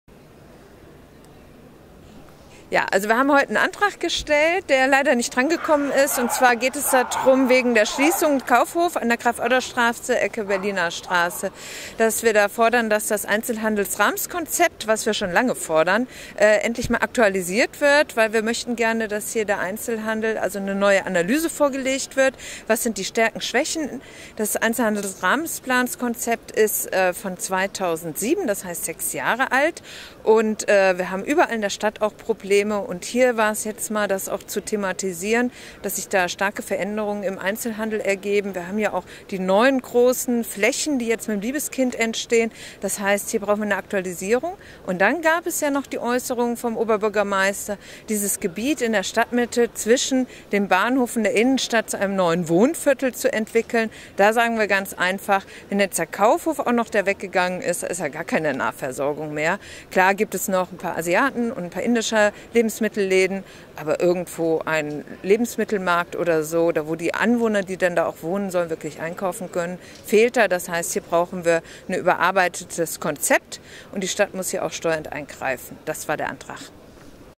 Ratsfrau Astrid Wiesendorf zum Einzelhandelsrahmenkonzept Düsseldorf
Videostatement zur Ratssitzung 11. Juli 2013